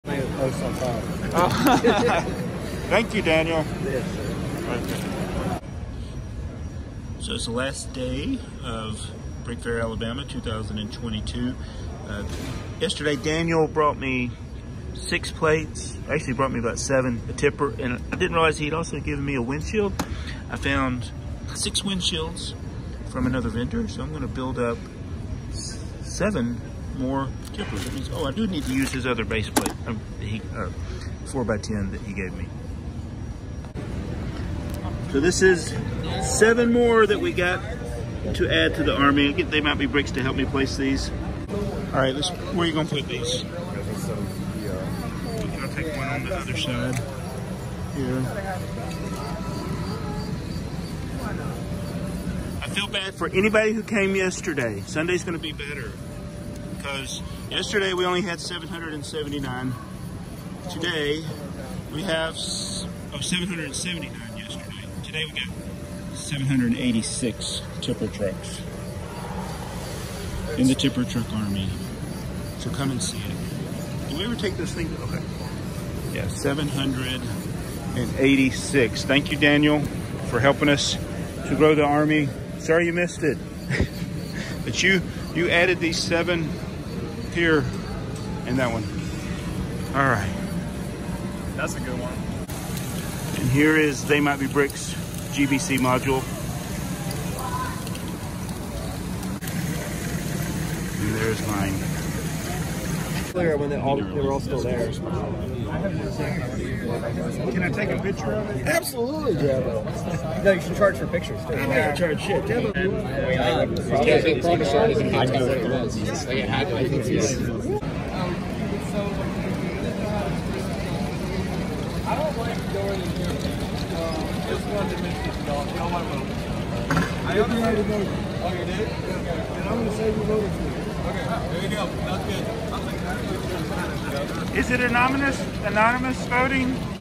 Sunday BrickFair Alabama 2022- LEGO Tipper Truck Army at 786
Thanks Daniel for the parts and others that have contributed. Found some more windshields and was able to add 7 for a grand total of 786

📭 Mail to:
BrickTsar
66 Fire Tower Rd NW #  264

➡️My eBay